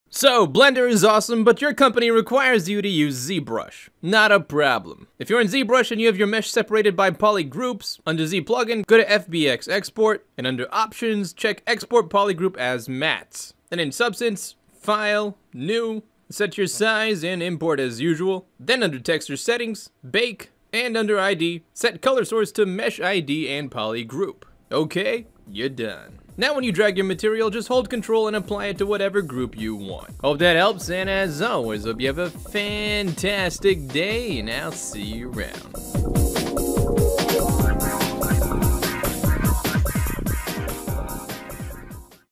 ZBrush Polygroups to Substance Painter ID's (FAST!!)
Here's the fastest way to turn your Polygroups into Texture ID's in Substance Painter!!